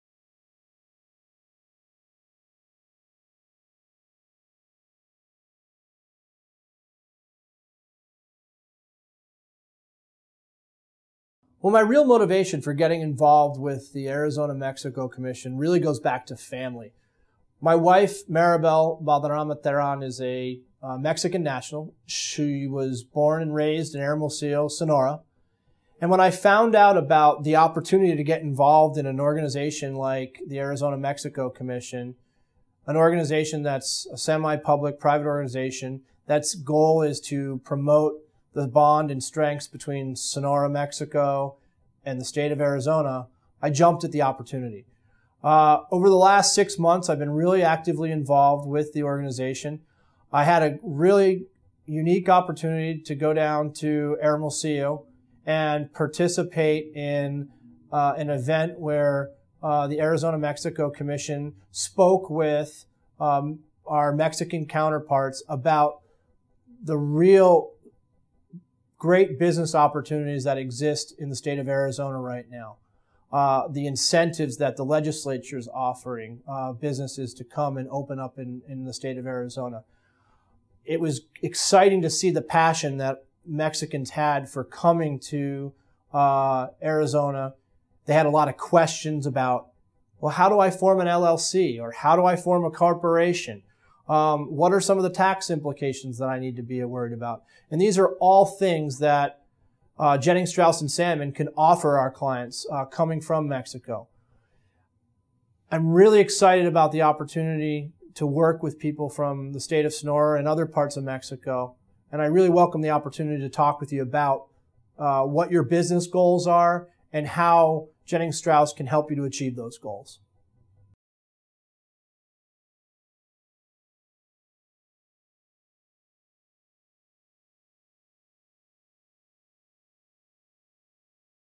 U.S. - Mexico Commercial Transactions
Jennings, Strouss & Salmon attorney Patrick F. Welch discusses his participation with the Arizona-Mexico Commission and how Jennings Strouss attorneys can assist clients with business transactions in Arizona.